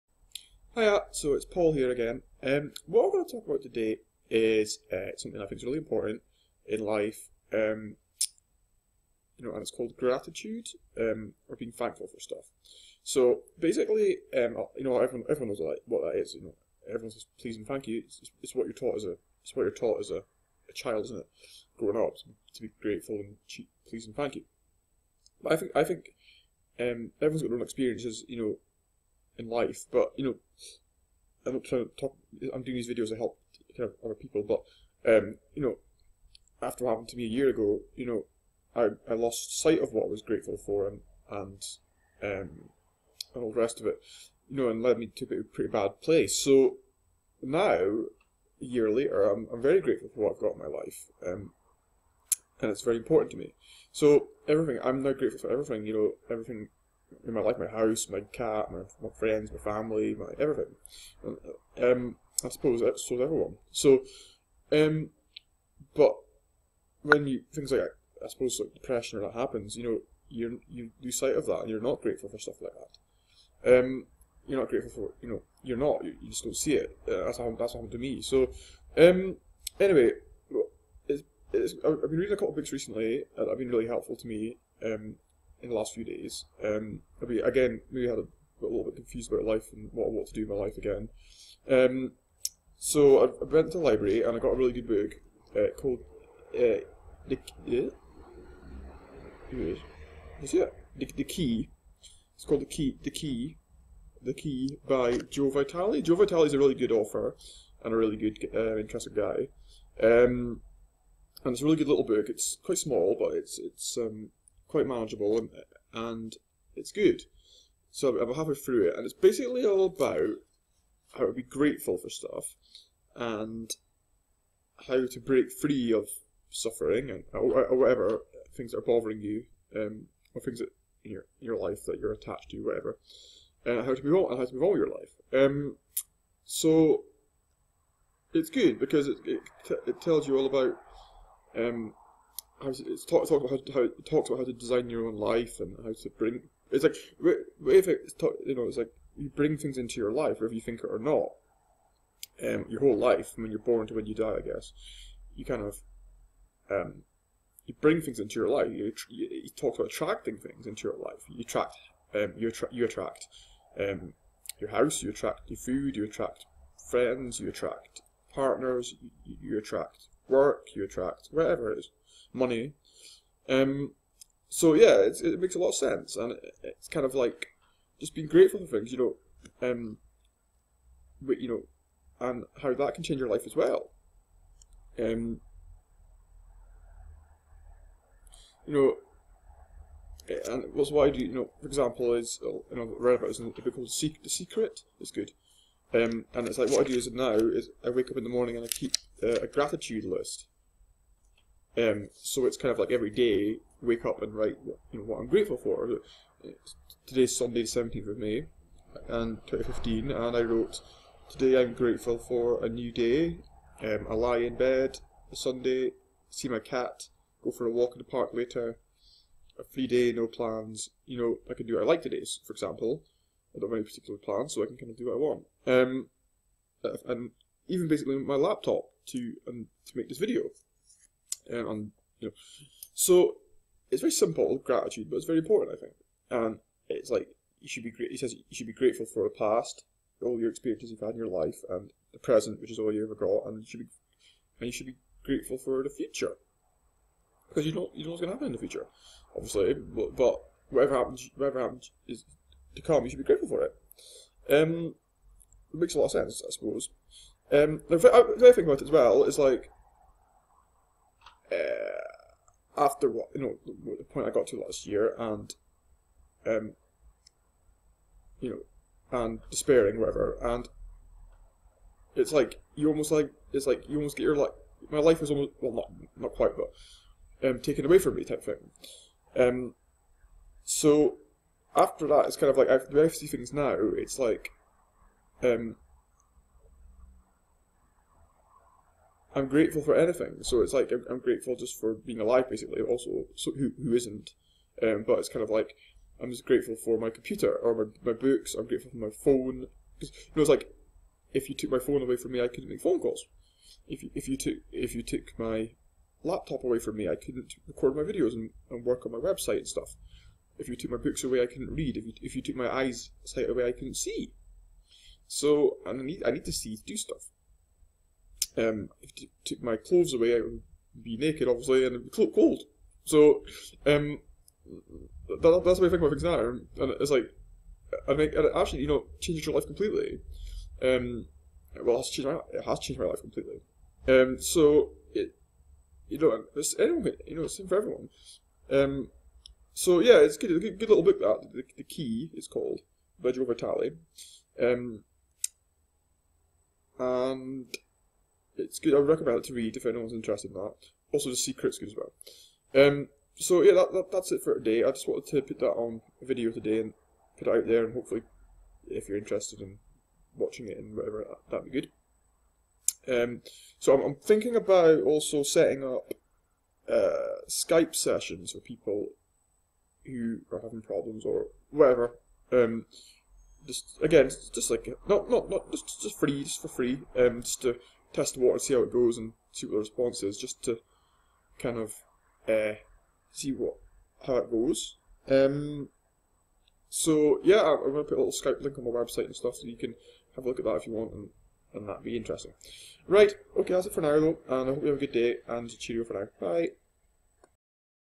Paul Brannan video Gratitude
In this video I discuss gratitude, which I feel is really important in life. Also I discuss a good book Ive been reading called The Key by Dr Joe Vitale. Hope you enjoy watching it! :)